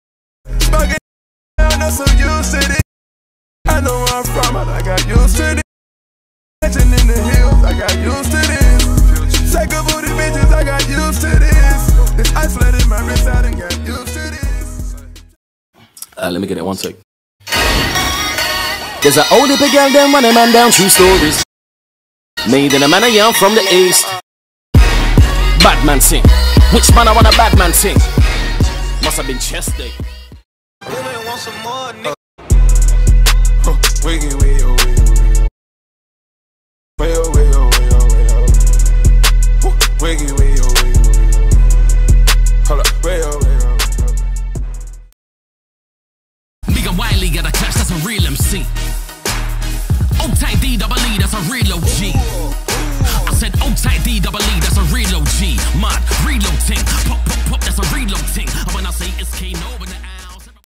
Lit Animation - Future v Kano.
An experimental piece and a simple execution using Photoshop and Illustrator to merge the four tracks listed.